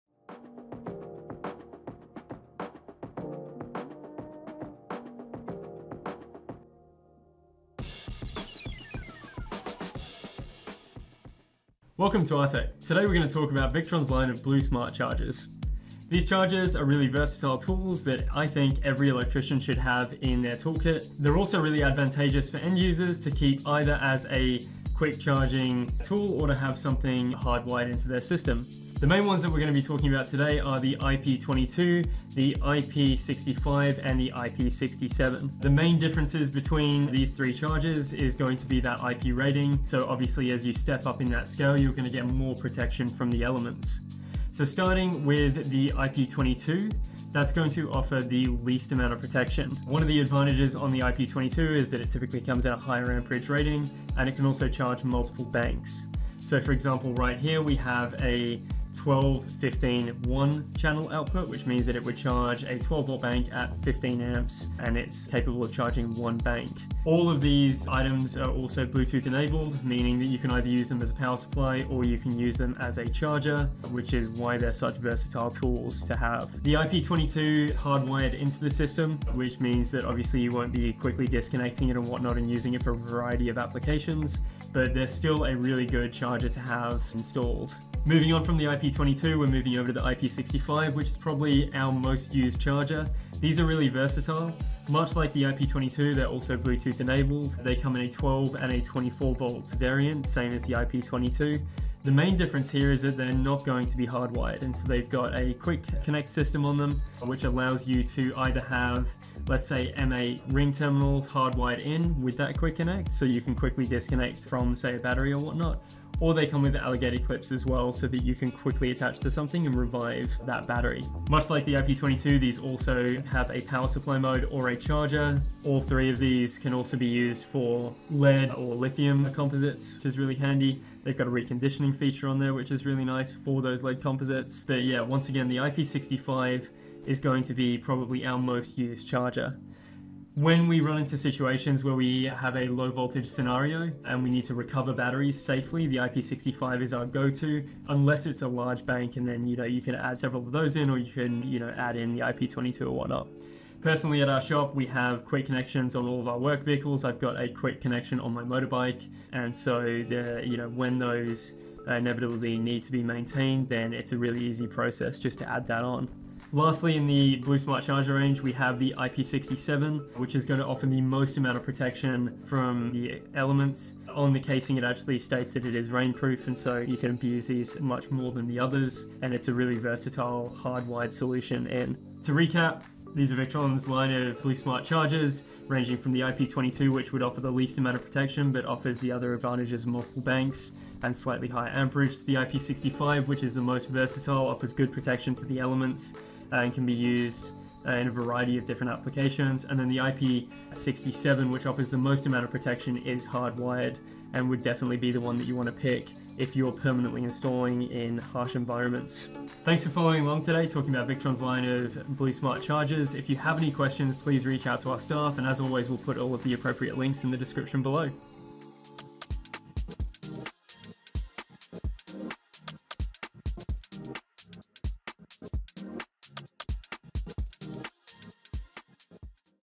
Victron Energy Smart Chargers | Overview & Basic Information
Welcome to our comprehensive overview of Victron Energy Smart Chargers, led by Victron ambassador and founder of Artek Energy, Sam Resier. In this video, Sam will walk you through the basic functions and key features of these intelligent chargers, making it easier for you to understand how Victron Energy Smart Chargers can optimize your battery charging process and enhance your overall energy management system.

Artek Energy is a trusted wholesale distributor of Victron products, dedicated to providing high-quality solutions for your energy needs.